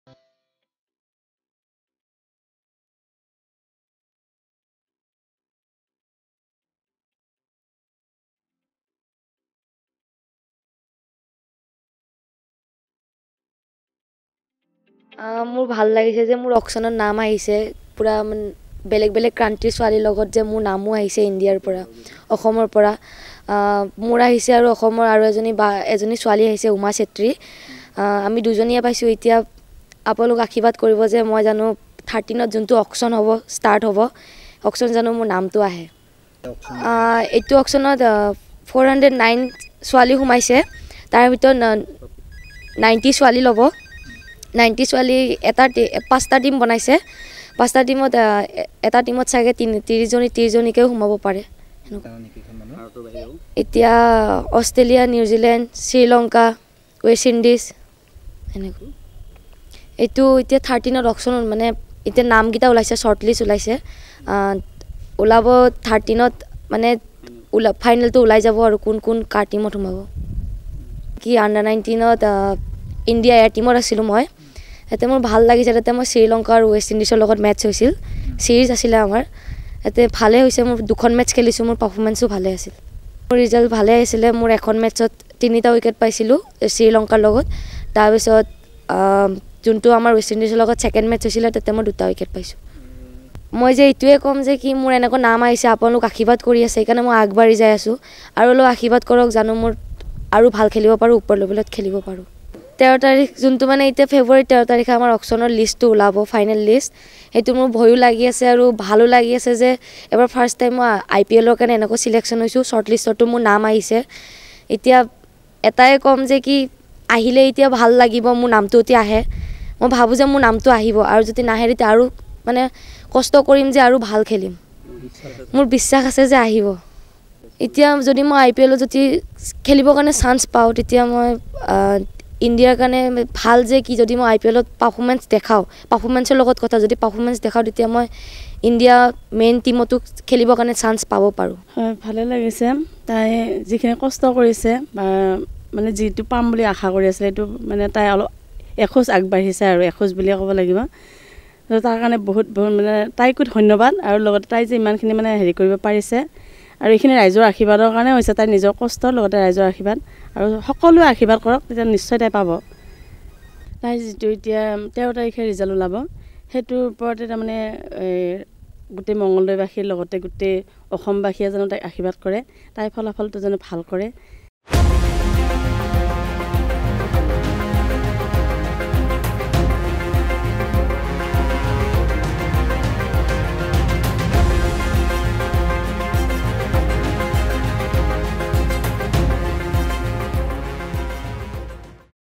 WPLAuction, অসমবাসীৰ মান ৰাখিলে মঙলদৈৰ জিণ্টিমণি কলিতাই
▶️অসমবাসীৰ মান ৰাখিলে মঙলদৈৰ জিণ্টিমণি কলিতাই। মুম্বাই ইণ্ডিয়ানছে ১০ লাখ টকাত ক্ৰয় কৰিলে অসমৰ ক্ৰিকেটাৰগৰাকীক।  মহিলা IPLৰ নিলামৰ পূৰ্বে কি কৈছিল জিণ্টিমণি কলিতাই?